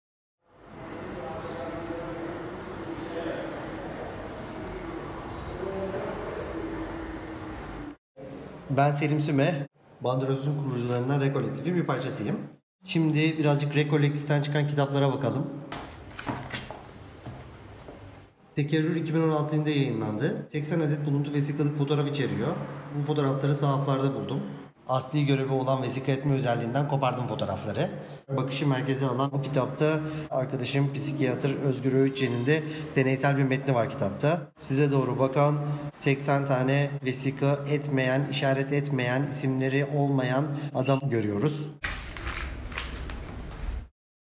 Selim Süme atölyesinde Bandrolsüz kolektifi ve kitabı Tekerrür'den bahsediyor
Bandrolsüz'ün kurucularından olan RecCollective'den Selim Süme, atölyesinde "Tekerrür" adlı kitabından bahsediyor.

Selim Süme ayrıca 29 Kasım 2018 – 05 Ocak 2019 tarihleri arasında Versus Art Project'te gerçekleşecek olan In The Blink Of An Eye isimli kişisel sergisinde, “fotoğrafı düşünmeyi” merkezine alırken makinanın imkanlarından yararlanarak “bakma eylemini” araştırıyor.